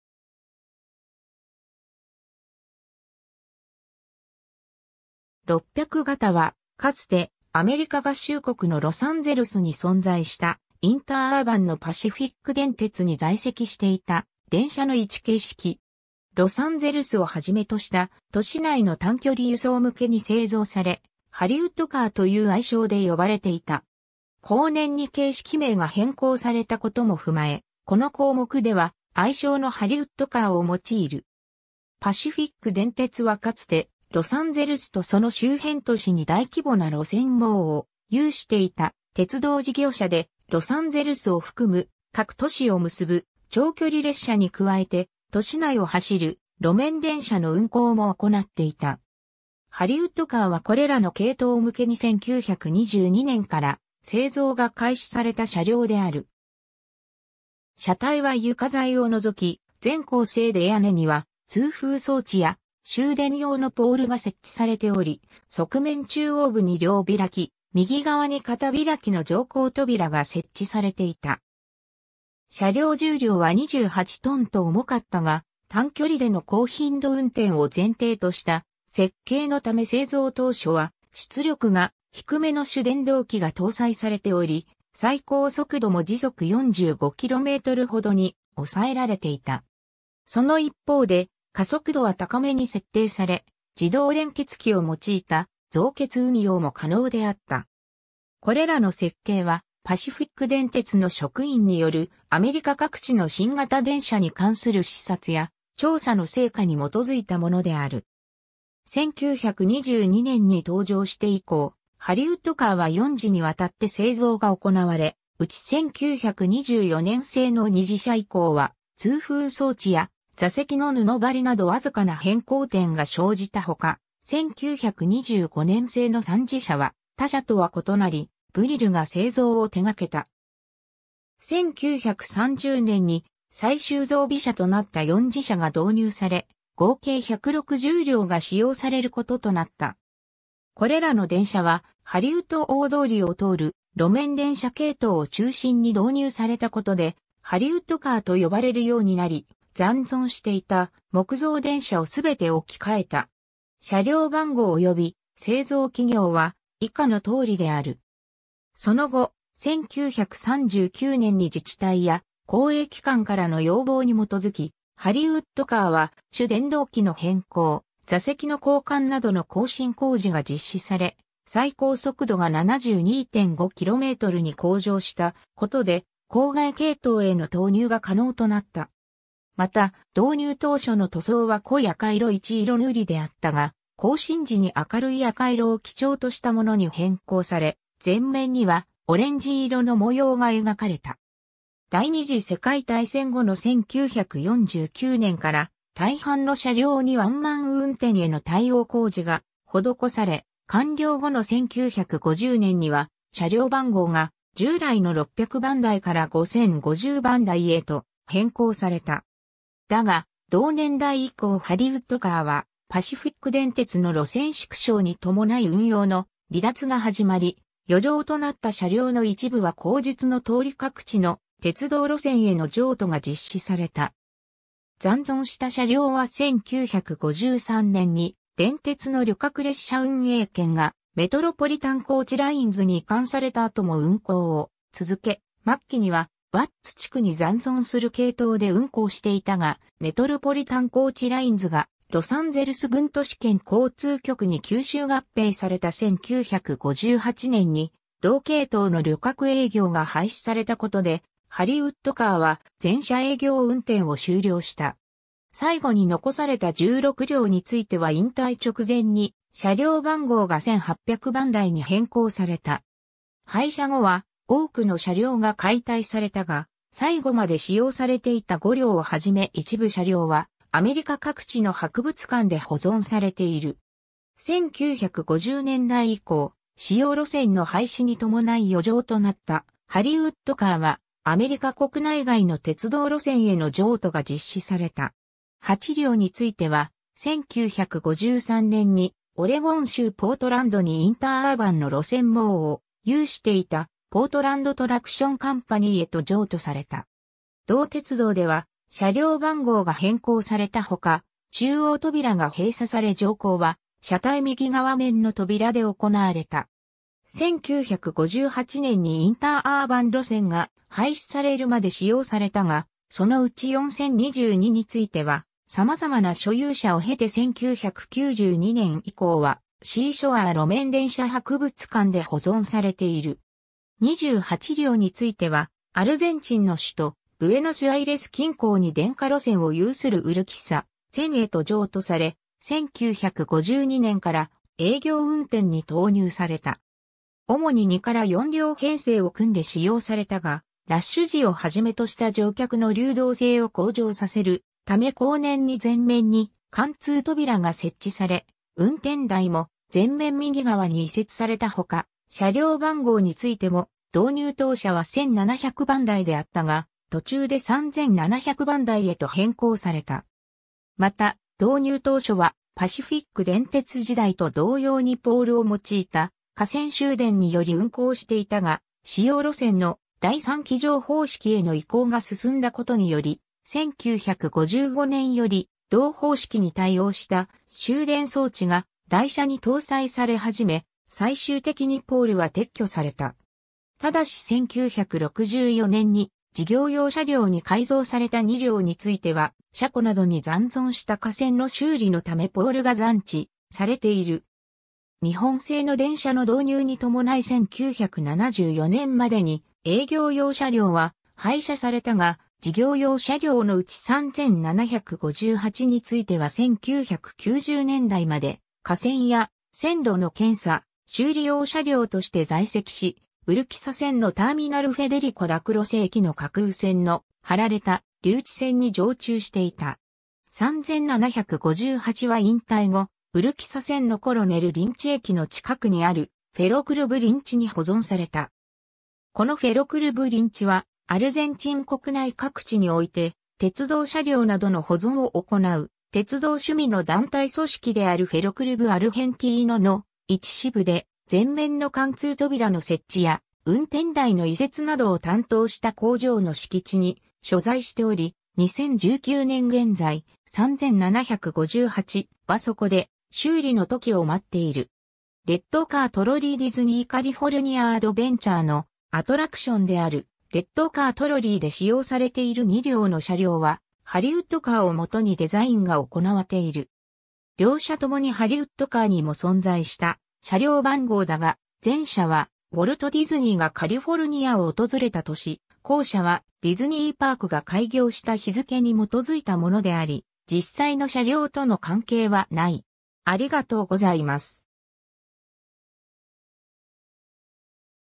パシフィック電鉄600形電車
600形（600 Class）は、かつてアメリカ合衆国のロサンゼルスに存在したインターアーバンのパシフィック電鉄（Pacific Electric Railway）に在籍していた電車の1形式。
ロサンゼルスを始めとした都市内の短距離輸送向けに製造され、"'ハリウッド・カー（Hollywood Car）"と言う愛称で呼ばれていた。
後年に形式名が変更された事も踏まえ、この項目では愛称の"ハリウッド・カー"を用いる。
 パシフィック電鉄はかつてロサンゼルスとその周辺都市に大規模な路線網を有していた鉄道事業者で、ロサンゼルスを含む各都市を結ぶ長距離列車に加えて都市内を走る路面電車の運行も行っていた。
ハリウッド・カーはこれらの系統向けに1922年から製造が開始された車両である。
 車体は床材を除き全鋼製で屋根には通風装置や集電用のポールが設置されており、側面中央部に両開き、右側に片開きの乗降扉が設置されていた。
車両重量は28 t（61,700 lbs）と重かったが、単距離での高頻度運転を前提とした設計のため製造当初は出力が低めの主電動機が搭載されており、最高速度も45 km/h（28 mph）程に抑えられていた。
その一方で加速度は高めに設定され、自動連結器を用いた増結運用も可能であった。
これらの設計は、パシフィック電鉄の職員によるアメリカ各地の新型電車に関する視察や調査の成果に基づいたものである。
 1922年に登場して以降、ハリウッド・カーは4次に渡って製造が行われ、うち1924年製の2次車（50両）以降は通風装置や座席の布張りなど僅かな変更点が生じた他、1925年製の3次車（50両）は他車（セントルイス・カー・カンパニー製）とは異なりブリルが製造を手掛けた。
1930年に最終増備車となった4次車（10両）が導入され、合計160両が使用される事となった。
これらの電車はハリウッド大通りを通る路面電車系統を中心に導入された事で「ハリウッド・カー」と呼ばれるようになり、残存していた木造電車を全て置き換えた。
車両番号および製造企業は以下の通りである。
 その後、1939年に自治体や公営機関からの要望に基づき、ハリウッド・カーは主電動機の変更、座席の交換などの更新工事が実施され、最高速度が72.5 km（45 mph）に向上した事で郊外系統への投入が可能となった。
また、導入当初の塗装は濃い赤色（マルーン）1色塗りであったが、更新時に明るい赤色を基調としたものに変更され、前面にはオレンジ色の模様が描かれた。
第二次世界大戦後の1949年から大半の車両にワンマン運転への対応工事が施され、完了後の1950年には車両番号が従来の600番台（600形）から5050番台（5050形）へと変更された。
 だが、同年代以降ハリウッド・カーはパシフィック電鉄の路線縮小に伴い運用の離脱が始まり、余剰となった車両の一部は後述の通り各地の鉄道路線への譲渡が実施された。
残存した車両は1953年に電鉄の旅客列車運営権がメトロポリタン・コーチ・ラインズに移管された後も運行を続け、末期にはワッツ地区に残存する系統（ワッツ線）で運行していたが、メトロポリタン・コーチ・ラインズがロサンゼルス郡都市圏交通局に吸収合併された1958年に同系統の旅客営業が廃止された事でハリウッド・カーは全車営業運転を終了した。
最後に残された16両については引退直前に車両番号が1800番台（1800形）に変更された。
 廃車後は多くの車両が解体されたが、最後まで使用されていた5両（南カリフォルニア鉄道博物館）を始め一部車両はアメリカ各地の博物館で保存されている 。
 1950年代以降、使用路線の廃止に伴い余剰となったハリウッド・カーはアメリカ国内外の鉄道路線への譲渡が実施された。
 8両については、1953年にオレゴン州ポートランドにインターアーバンの路線網を有していたポートランド・トラクション・カンパニー（Portland Traction Company）へと譲渡された。
同鉄道では車両番号が変更された他、中央扉が閉鎖され乗降は車体右側面の扉で行われた。
1958年にインターアーバン路線が廃止されるまで使用されたが、そのうち4022（旧：680→）については様々な所有者を経て1992年以降はシーショアー路面電車博物館で保存されている。
 28両についてはアルゼンチンの首都・ブエノスアイレス近郊...